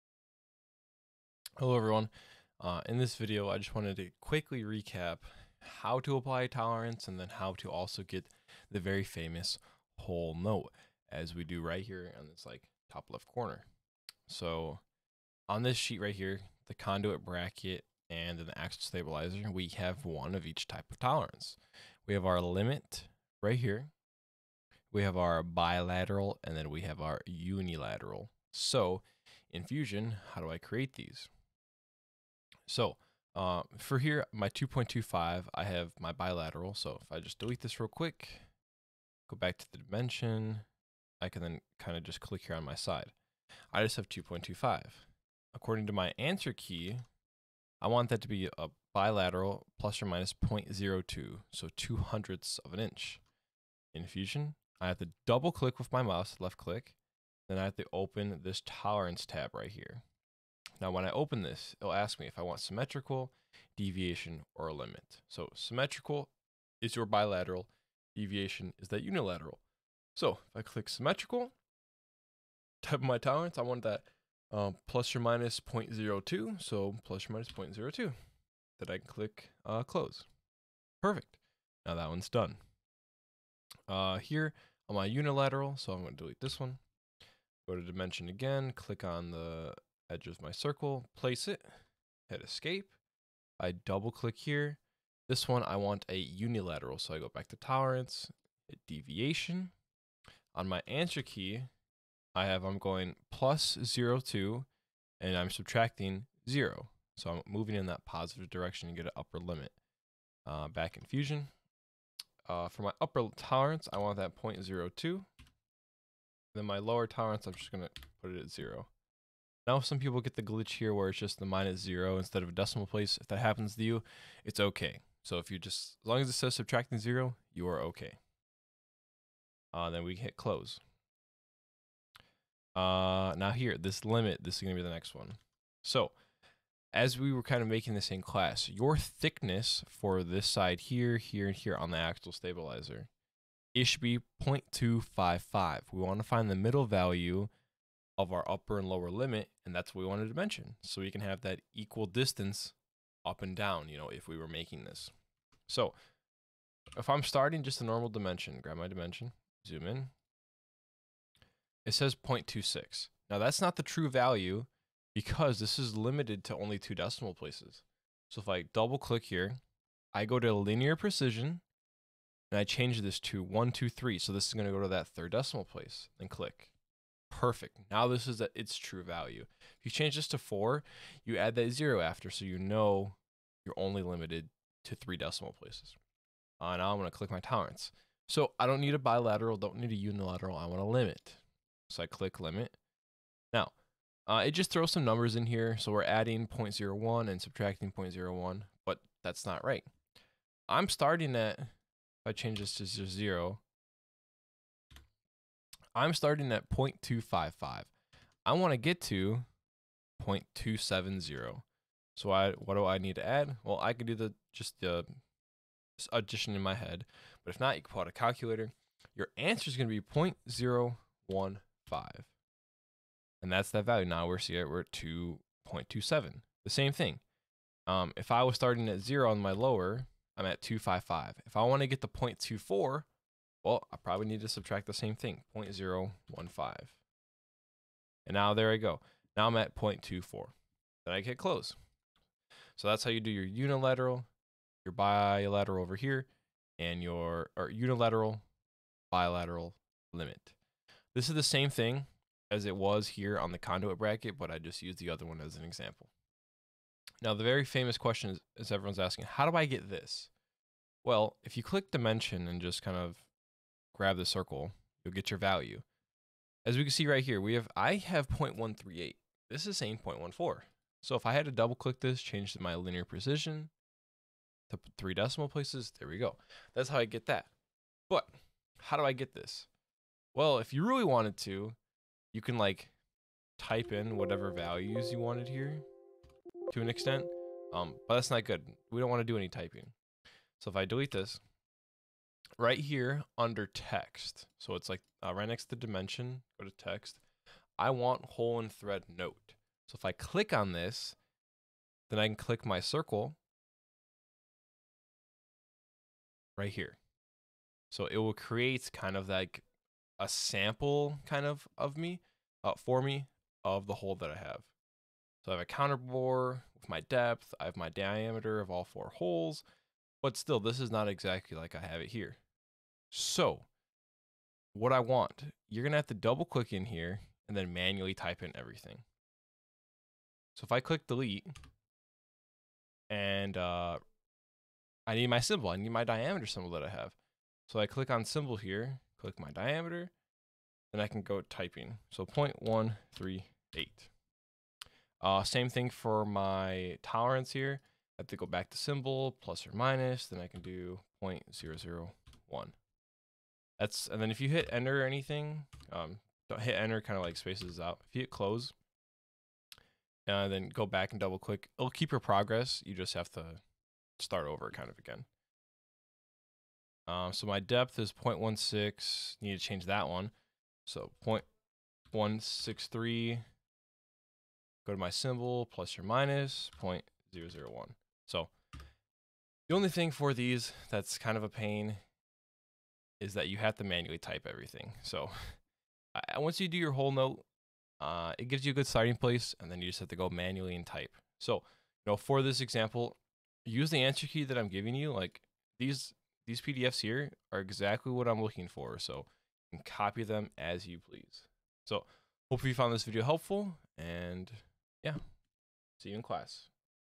Fusion | How to place Tolerances and Hole/Thread Notes on a Drawing Sheet
PLTW IED
MSHS & MWHS